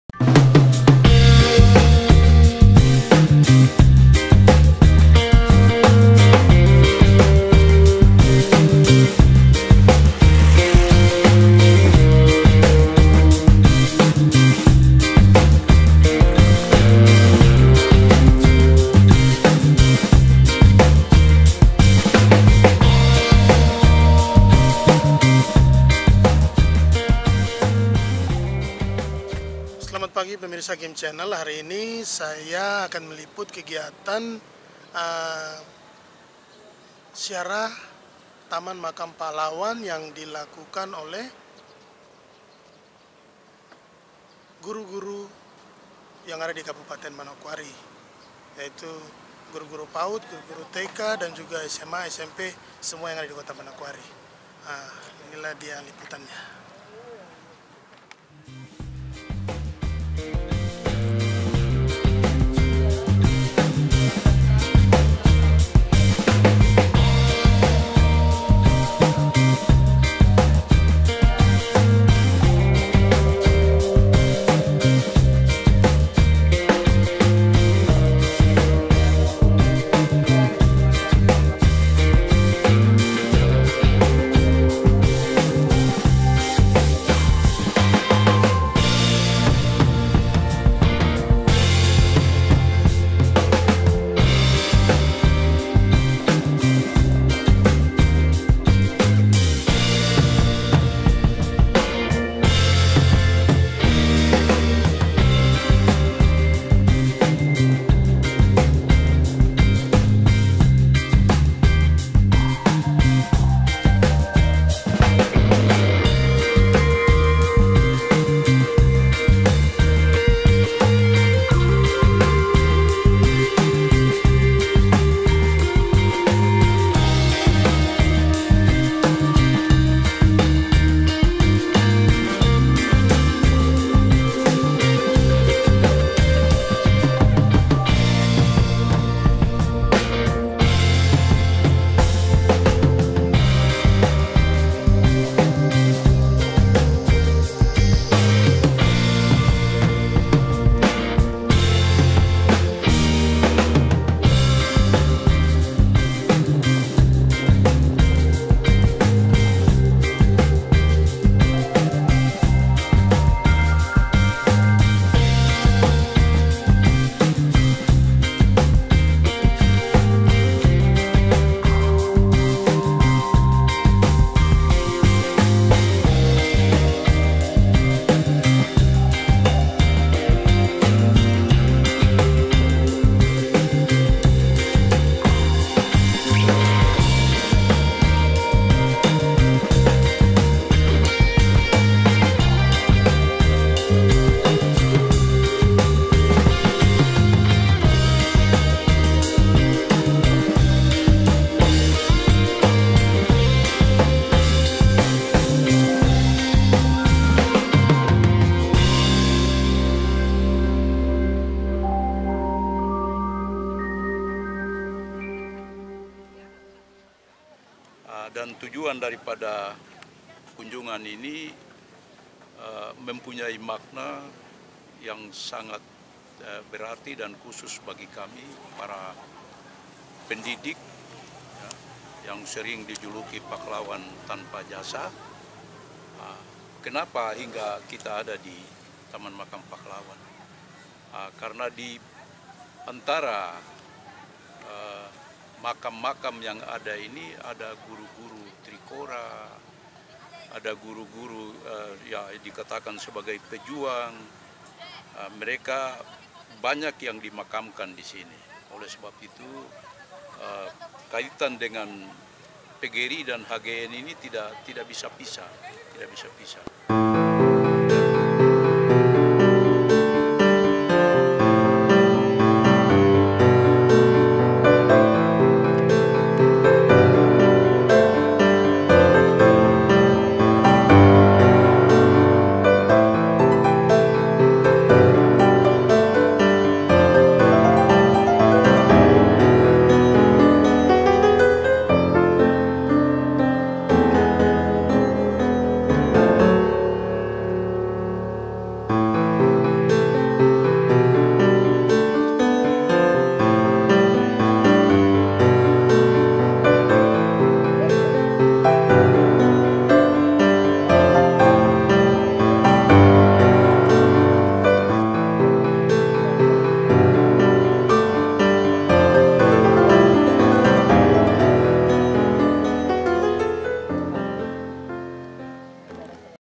## ZIARAH TAMAN MAKAM PAHLAWAN ## PERINGATAN HUT PGRI KE 74 DAN HARI GURU NASIONA##
PEMIRSA PERINGATAN HUT PGRI  KE 74 DAN JUGA HARI GURU NASIONAL TAHUN 2019 / KEPALA – KEPALA SEKELAH / MELAKSANAKAN ZIARAH KUBUR / DI TAMAN MAKAM PAHLAWAN TRIKORA MANOWKARI /
ZIARAH KUBUR / DI TAMAN MAKAM PAHLAWAN TRIKORA MANOKWARI / DALAM MOMEN HUT PGRI KE 74 DAN PERINGATAN  HARI GURU NASIONAL TAHUN 2019 / DI PILIH SEBAGAI / BENTUK PENGHORMATAN / ATAS JASA  / PARA PEJUANG DALAM MEREBUT KEMERDEKAAN / DARI PENJAJAH DAN JUGA KEMERDEKAAN DARI KEMISKINAN DAN KEBODOHAN //